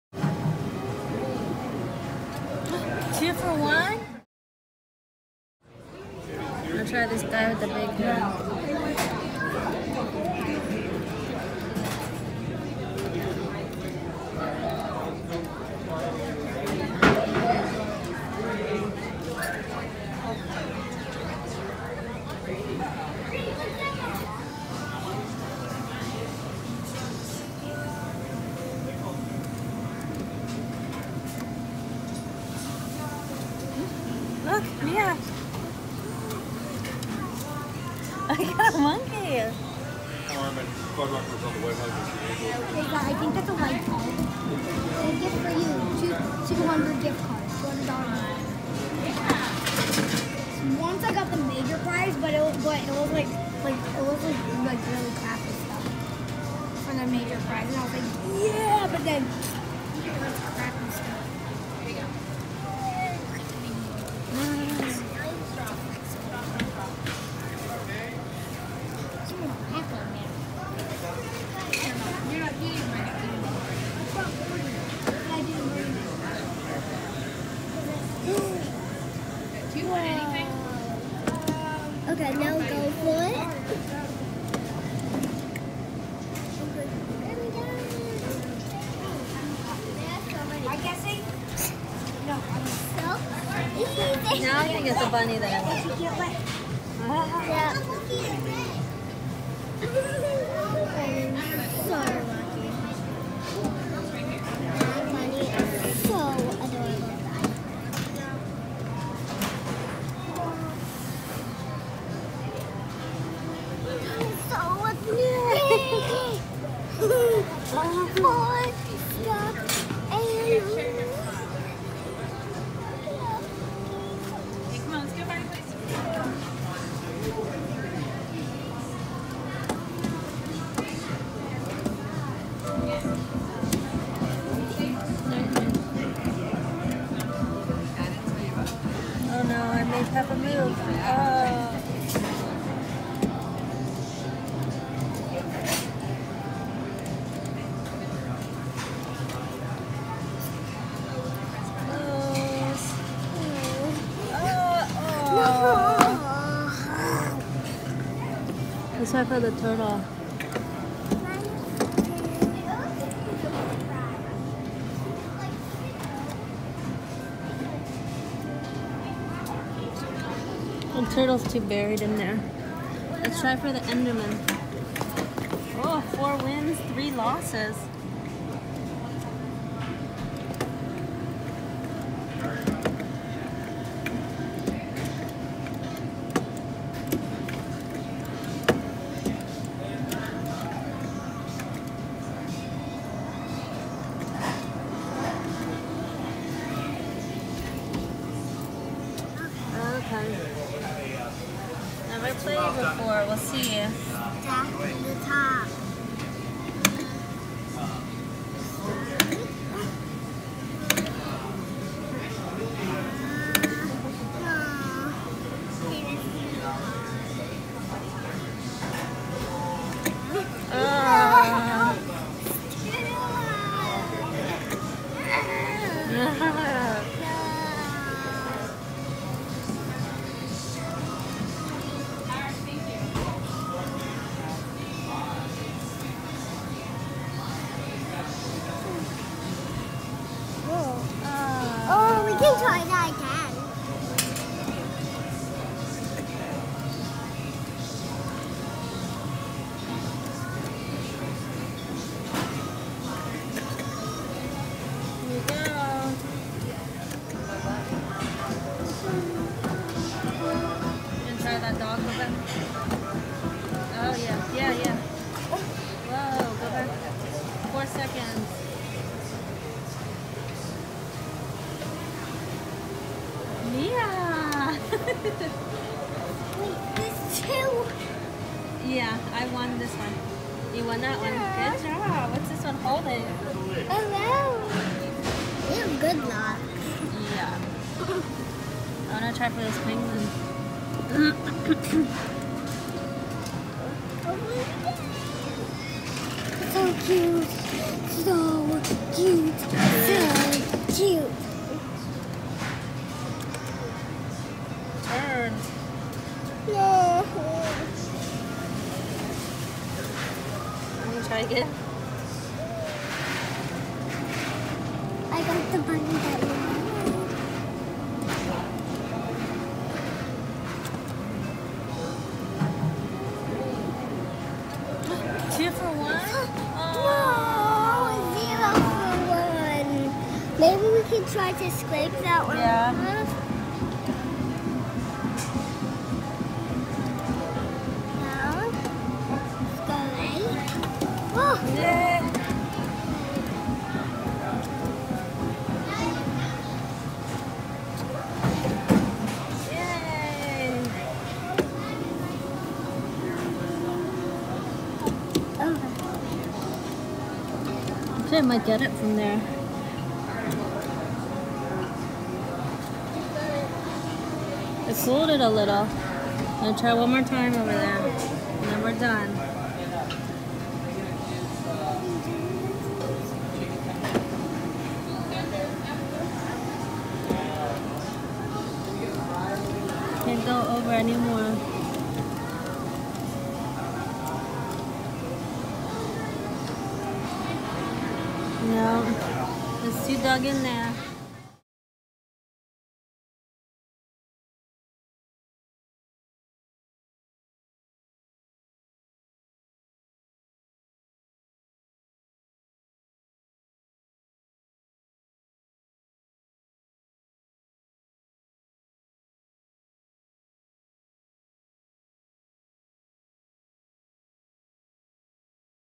Claw Machine Wins ~ Claw Machine Fun #29 ~ Toy House ~ Plush Wins ~ 2 for 1?
Today we did well at the claw! We were able to win a bunch of plush! Check out what we win!! I hope you enjoy this video!!

Surprise Eggs and Toy Fun!
Lawn, PA 17041